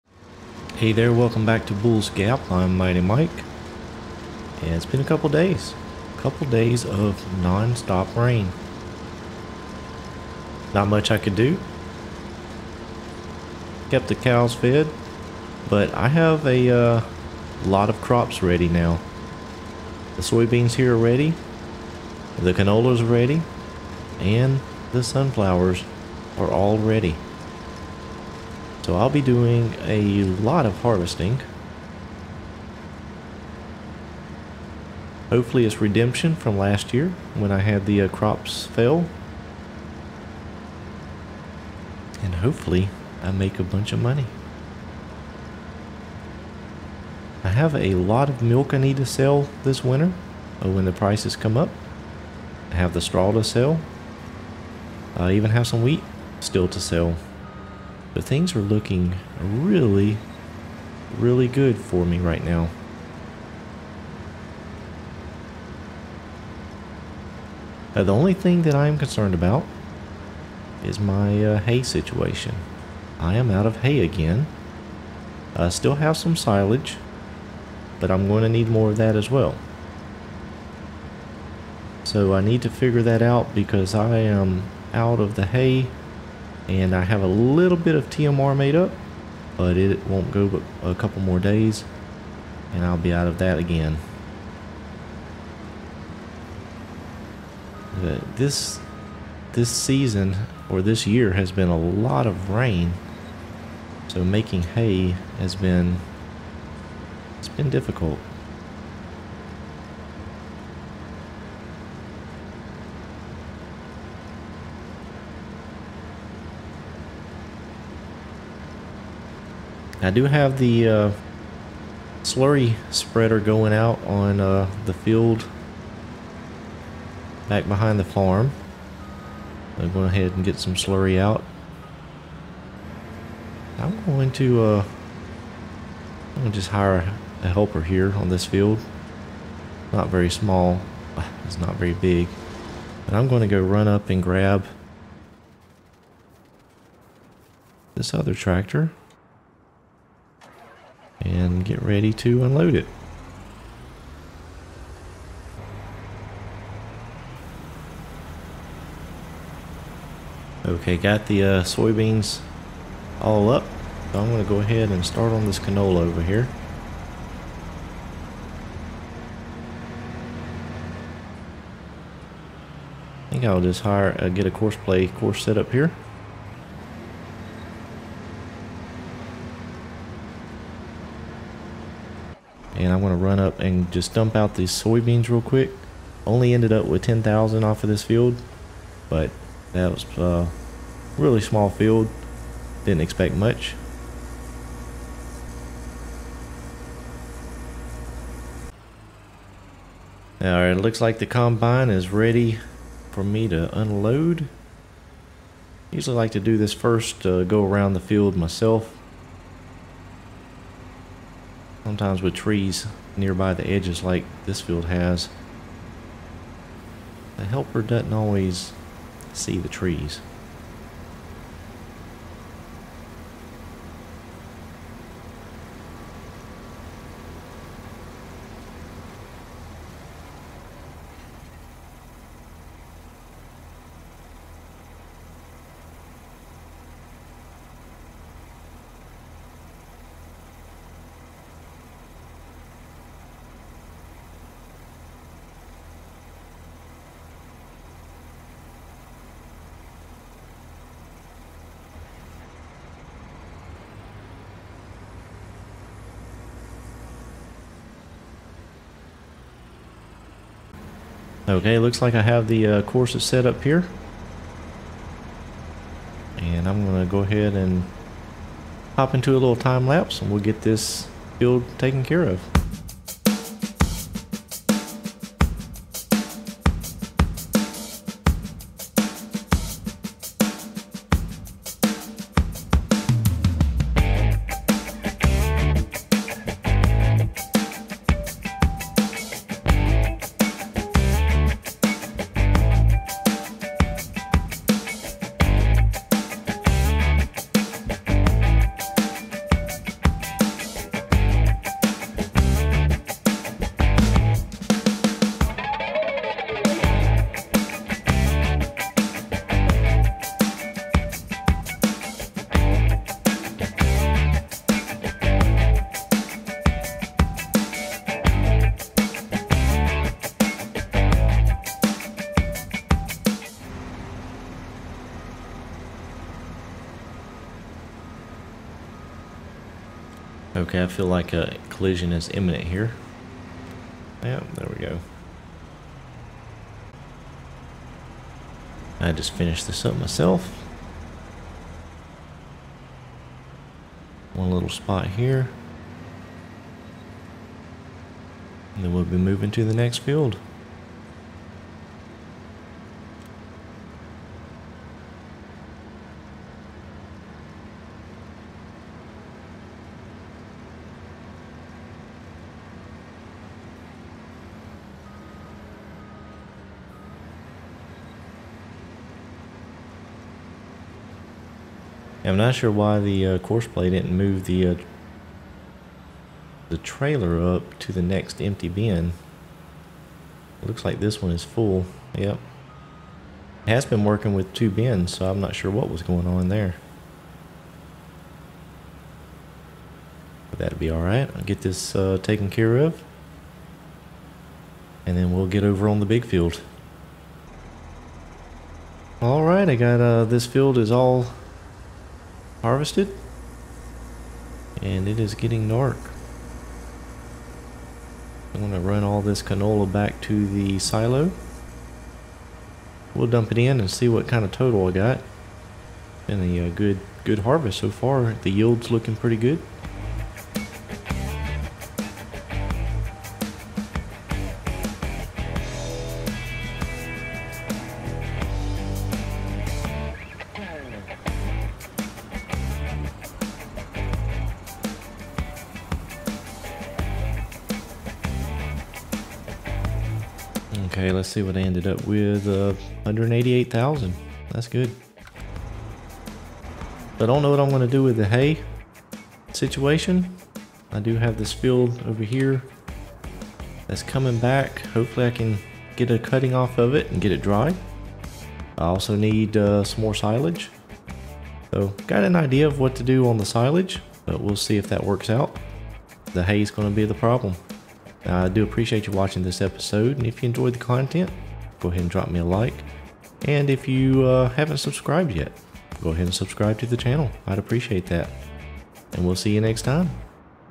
Ripped through the Canola | Bulls Gap, TN | 20
Welcome to this episode, here at Bulls Gap, TN (by Rich Acres Modding). I blitz through the canola fields and get the grain put into storage.

If you have any feedback or comments, please, leave them below.